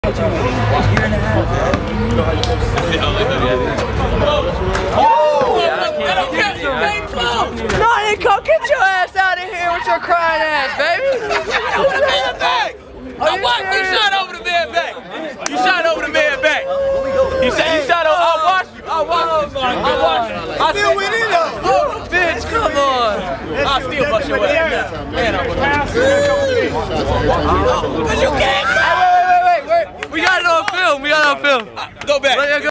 Mac Miller - Crazy Basketball Shot - Pittsburgh, PA
Mac Miller makes a crazy basketball shot before his show in Pittsburgh, PA on 7-12-13.  Filmed by Ben Fredette